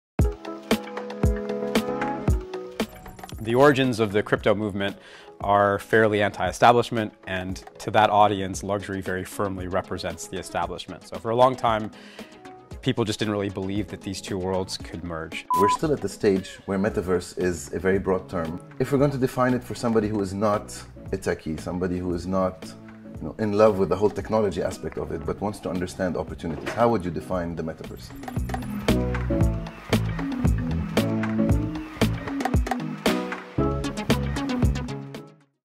inbold podcast Season 2 Episode 3, now live!
What is Web3 and the metaverse? And how is the fashion and luxury industry moving into this space? Hear it now from Shashi Menon!